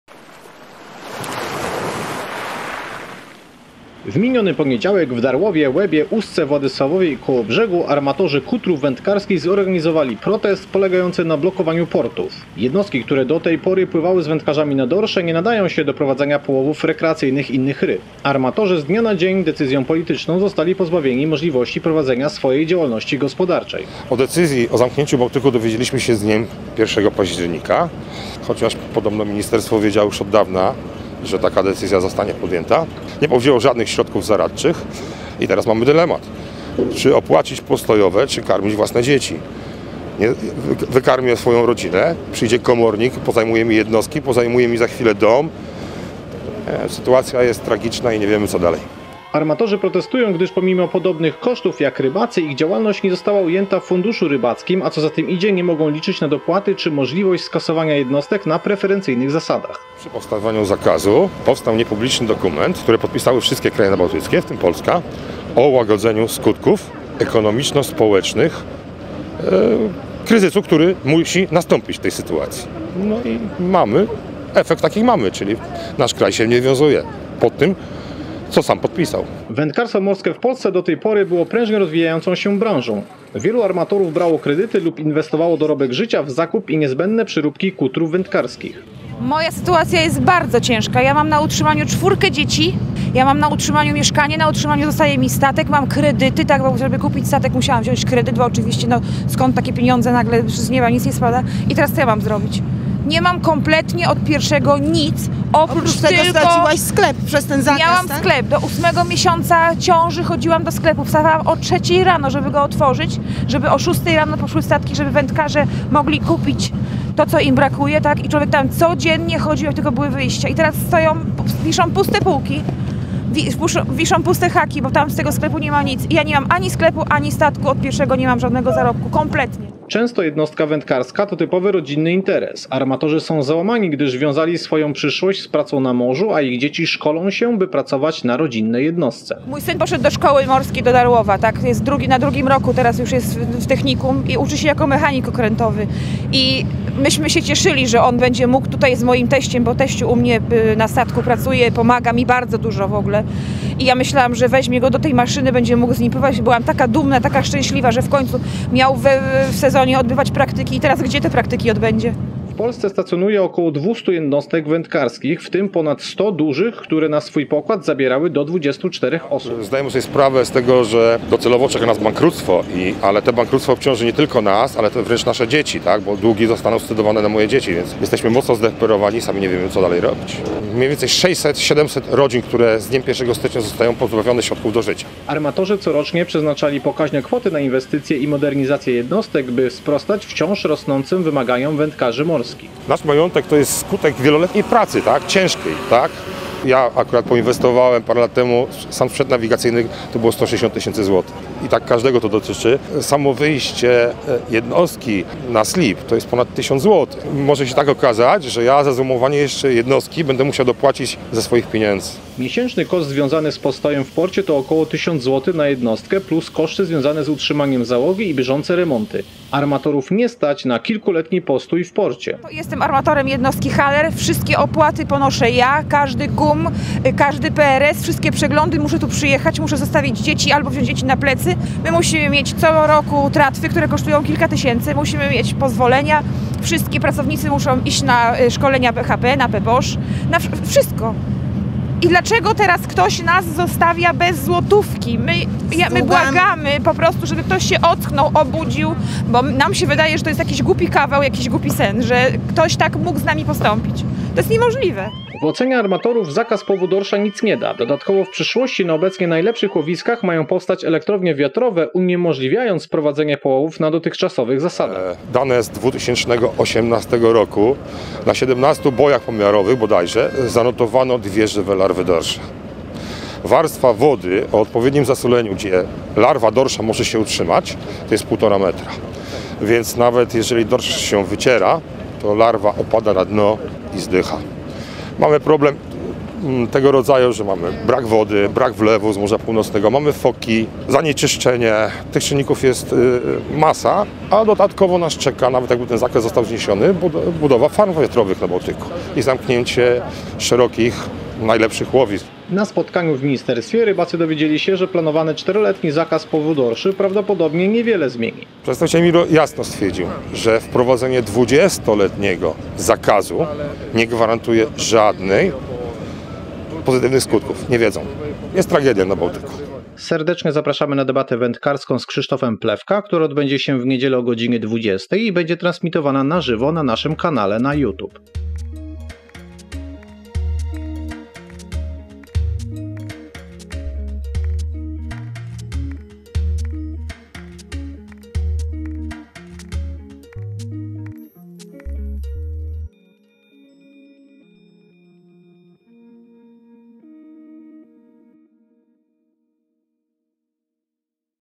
MorskaTV ➤Blokada portu - wywiad z armatorami kutrów wędkarskich
W miniony poniedziałek w Darłowie, Łebie, Ustce, Władysławowie i Kołobrzegu armatorzy kutrów wędkarskich zorganizowali protest polegający na blokowaniu portów. Jednostki które do tej porty pływały z wędkarzami na dorsze nie nadają się do prowadzenia połowów rekreacyjnych innych ryb. Armatorzy z dnia na dzień, decyzją polityczną zostali pozbawieni możliwości prowadzenia swojej działalności gospodarczej.

Ja poświęcam swój czas ale potrzebuje Twojego wsparcia by móc opłacić koszty związane z dojazdami i eksploatacją sprzętu.